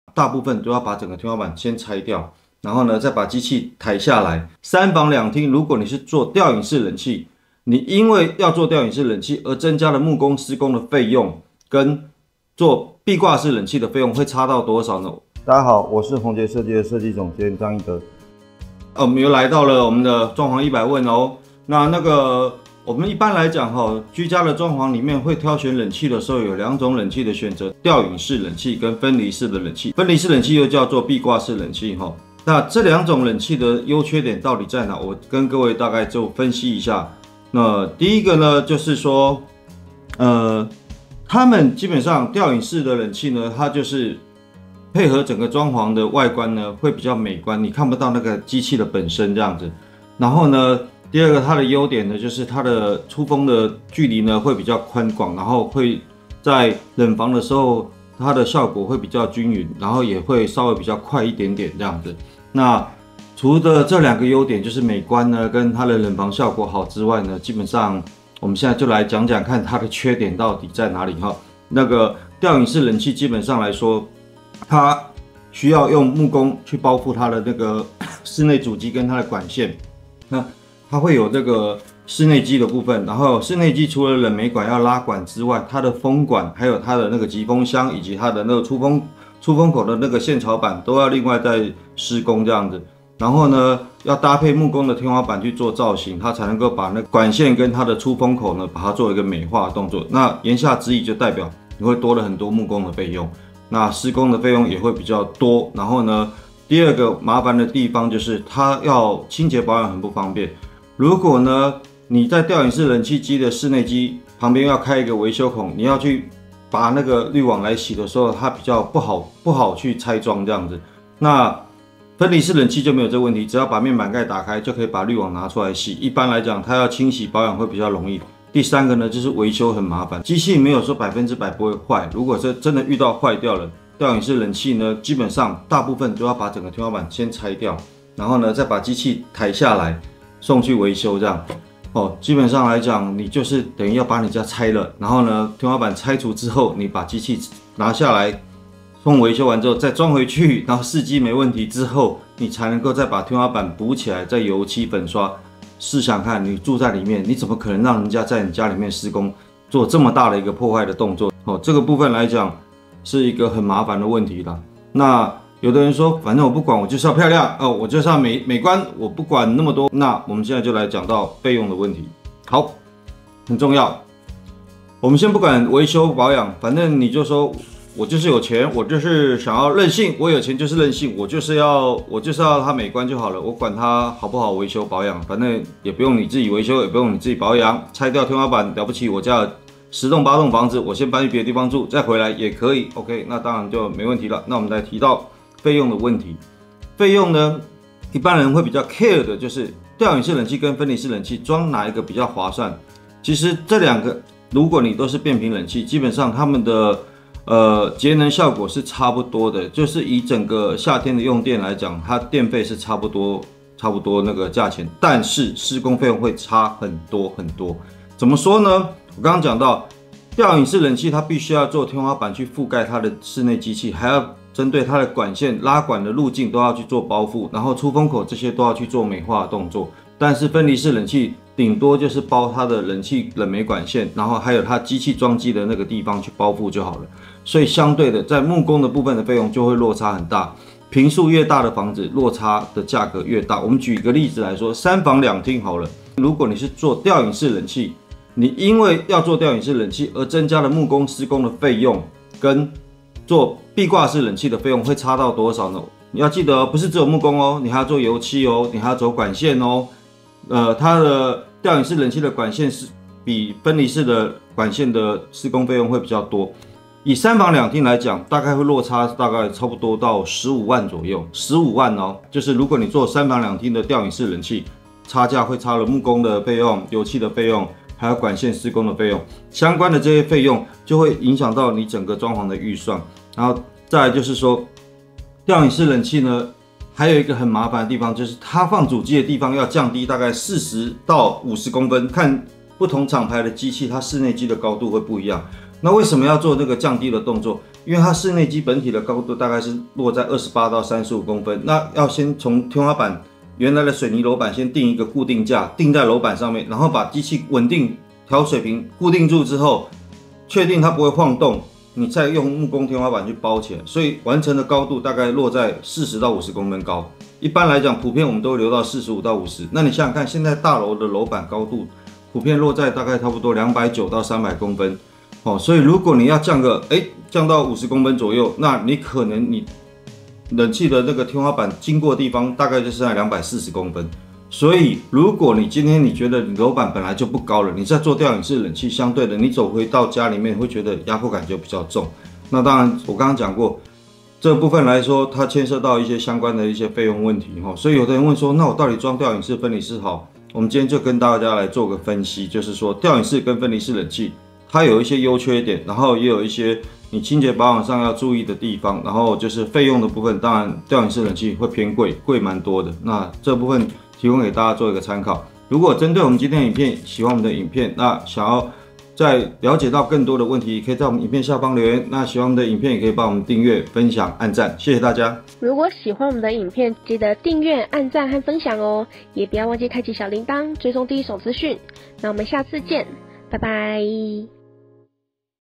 冷氣維修要拆天花板？｜吊隱式?壁掛式?有哪些優缺點?後續又該怎麼保養？設計師分析與推薦| 室內設計100問
| 室內設計100問 |
冷氣到底要選哪種好?只有看到出風口那種是?
原來牽扯到的裝潢有那麼多啊?
後續的清潔保養也是一大學問
就讓我們透過影片來了解吧~

☆若是對裝潢有任何問題☆
⬇歡迎在評論區提出⬇
德哥都會一一解答喔！

 台北｜新北｜桃園｜新竹｜

💡老屋不新 鴻杰有心
 陪你看房 用最合理的價格，為您打造專屬幸福空間💡